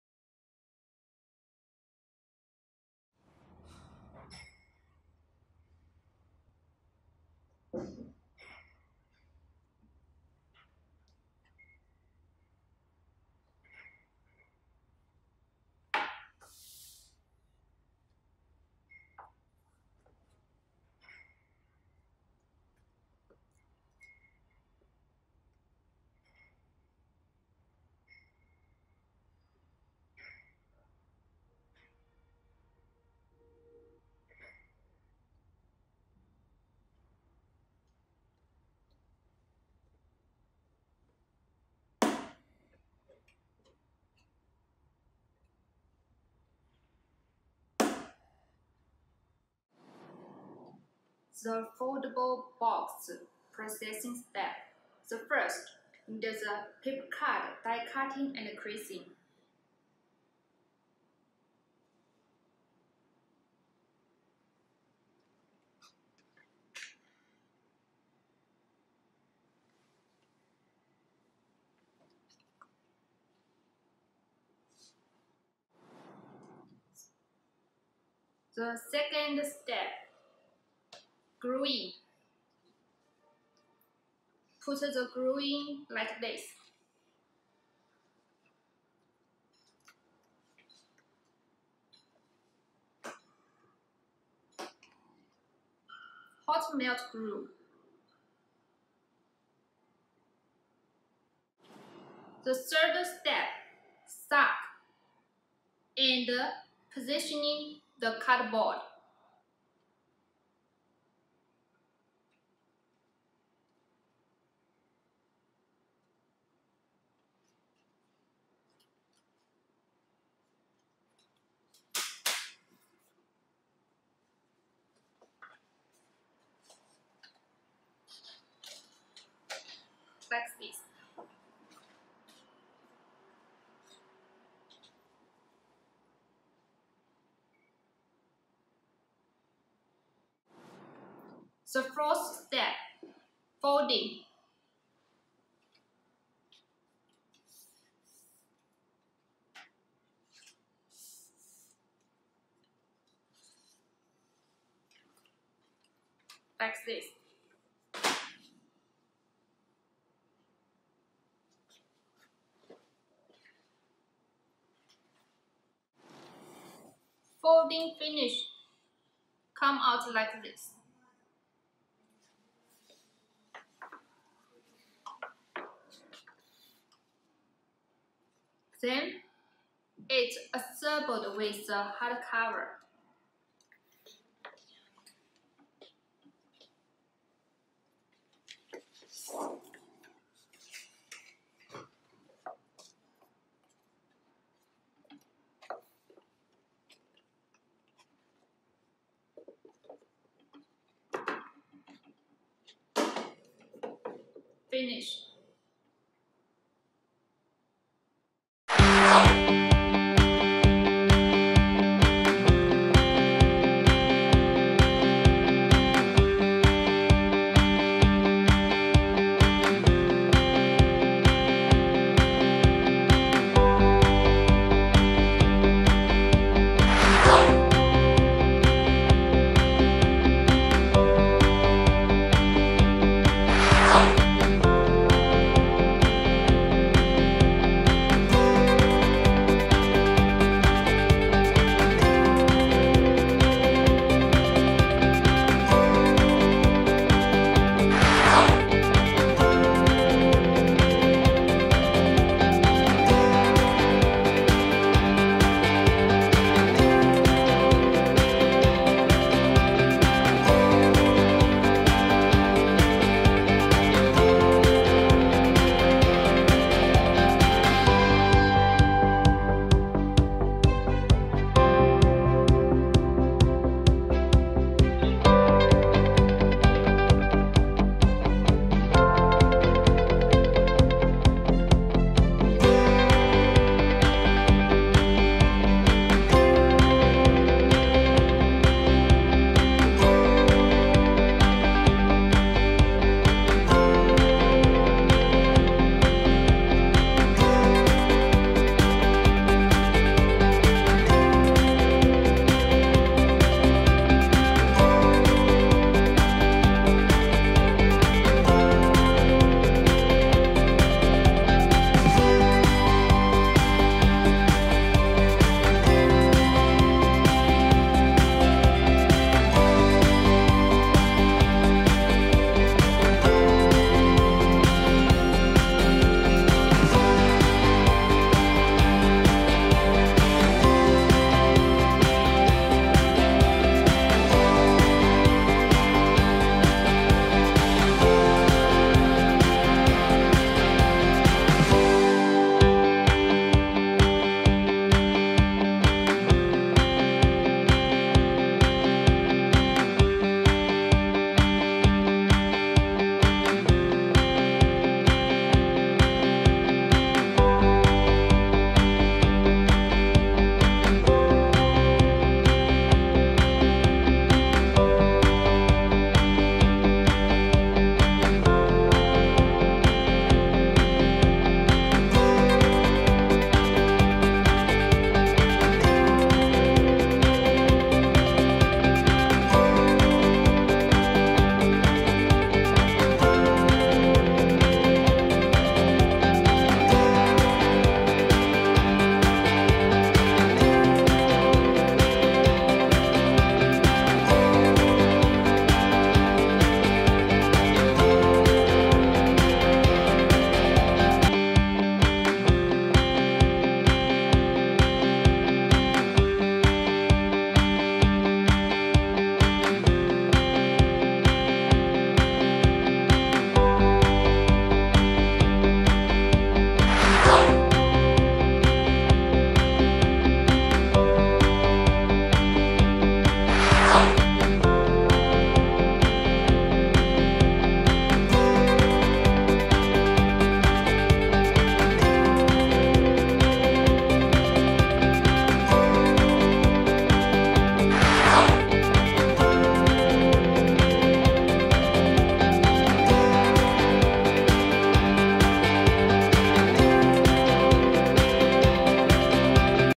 The Line to Make Collapsible Rigid Boxes by Mosun
Collapsible Rigid Boxes easy to storage, save warehousing fee and shipping fee.
A new design will start a new era in luxury packaging, less original material and less steps to a gift box.

Mosun with the fully automatic machine line to produce such fordable box.

Welcome to join us and develop with us together:
Wechat:Miamosuns008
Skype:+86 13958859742